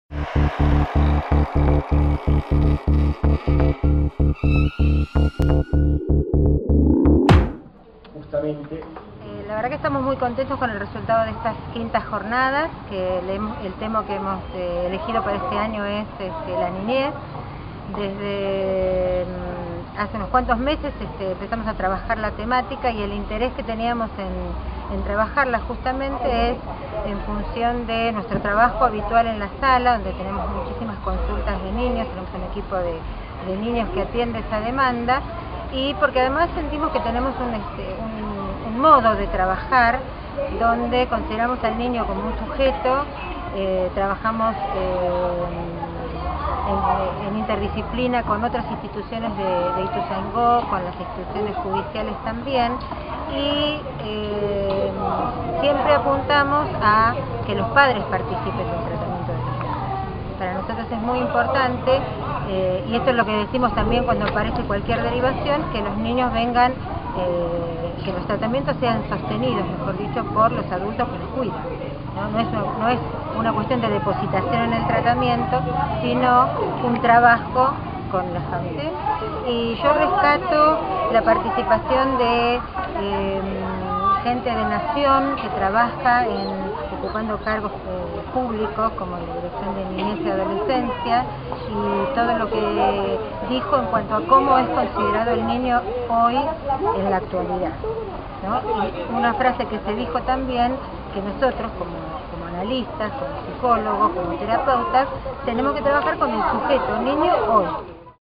La 5º jornada de salud mental se desarrolló con éxito
La Secretaría de Salud a través del Equipo de Salud Mental de la Unidad Sanitaria Villa Las Naciones realizó por quinto año consecutivo la Jornada de Salud Mental "Acerca de la niñez: síntoma, subjetividad e historia", en las instalaciones del Centro Deportivo y Recreativo La Torcaza, ubicado en Brandsen y Pringles.